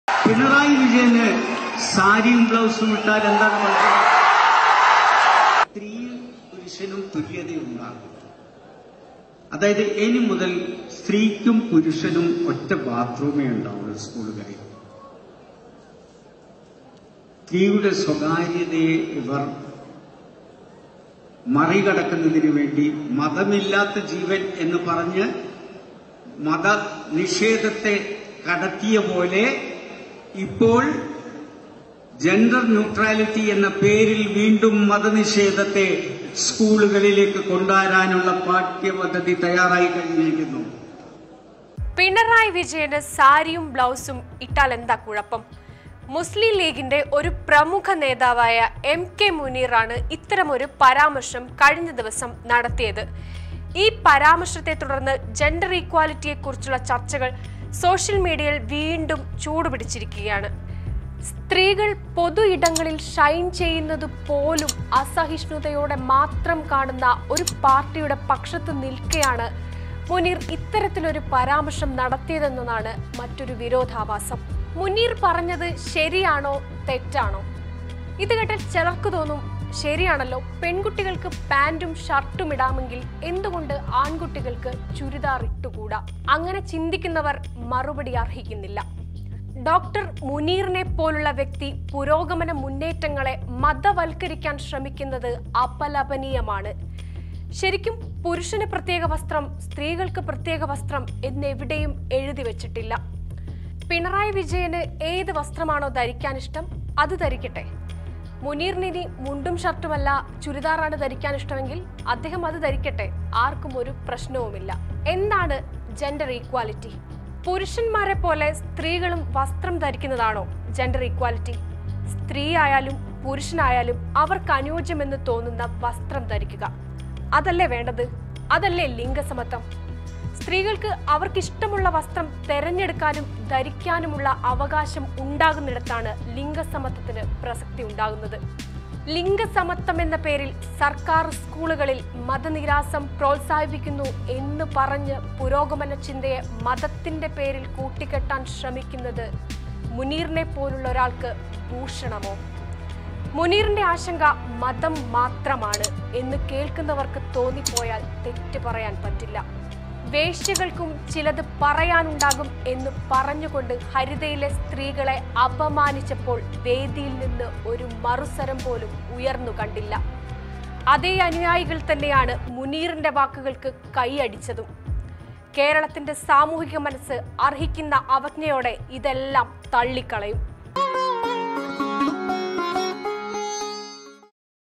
പിണറായി വിജയൻ സാരി ഉടുക്കണോ?
പുരുഷൻമാരെ പോലെ സ്ത്രീകളും വസ്ത്രം ധരിക്കുന്നതാണോ ലിംഗ സമത്വം?